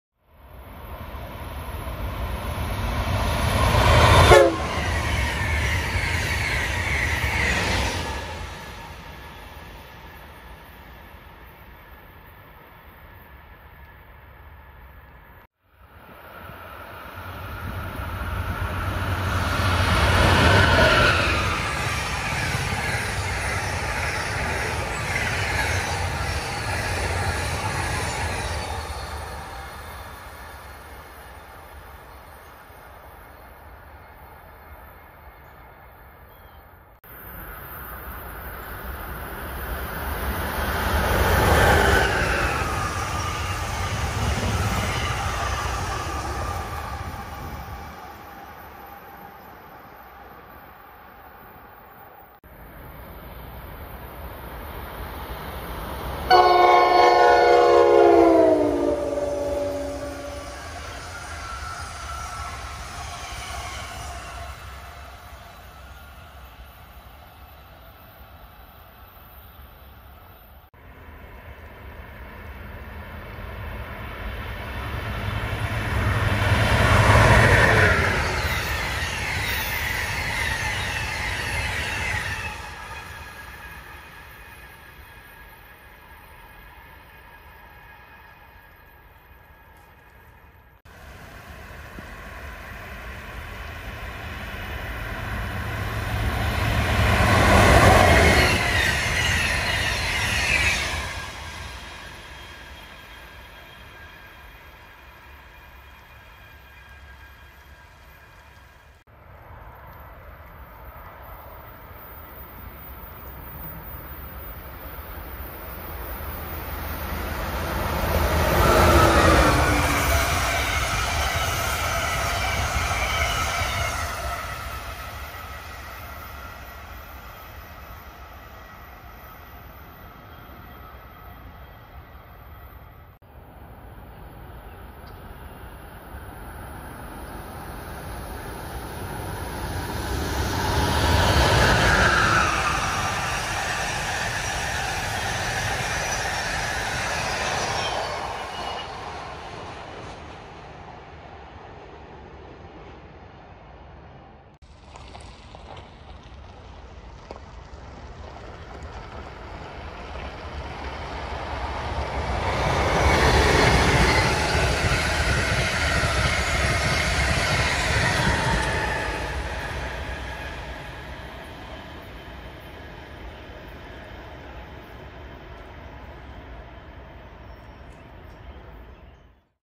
Amtrak & NJ Transit Northeast Corridor [NEC] Action @ Deans & MIDWAY (5/3/21)
From about 1530 to 1715. Enjoy =)